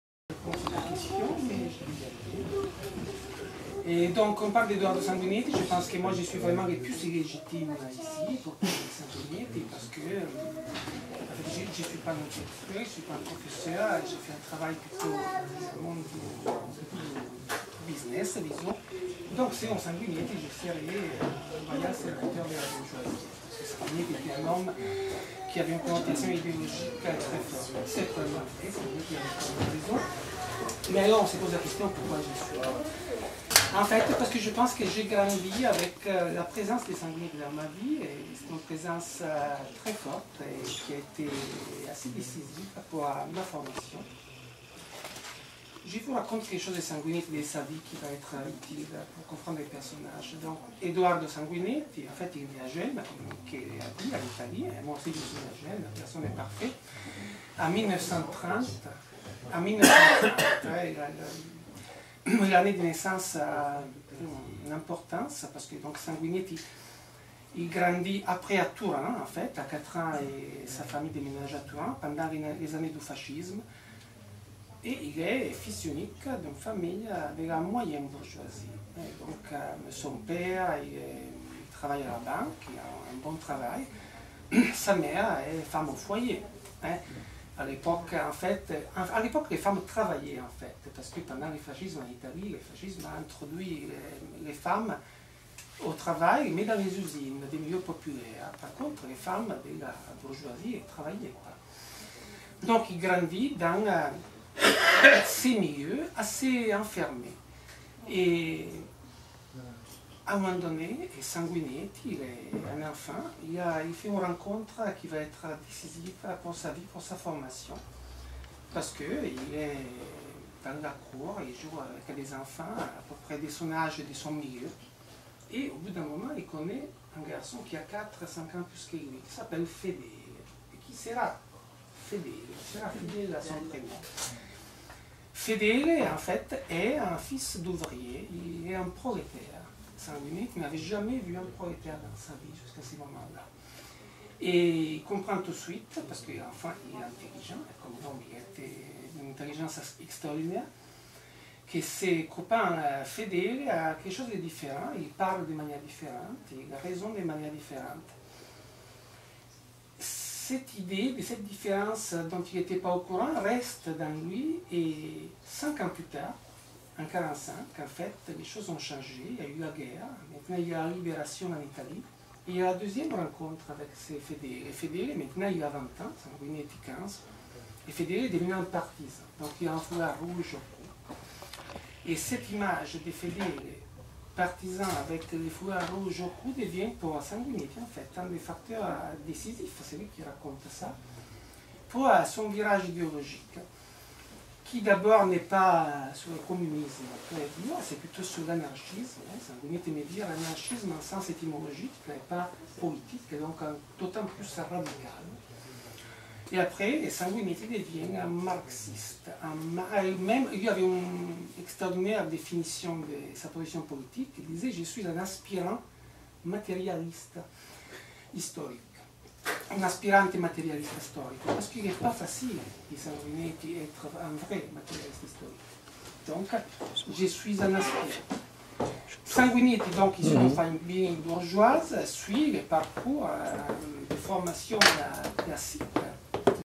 Soirée hommage à Edoardo Sanguineti à La Libreria à Paris
Où l'on écoute attentivement Maurizio Puppo...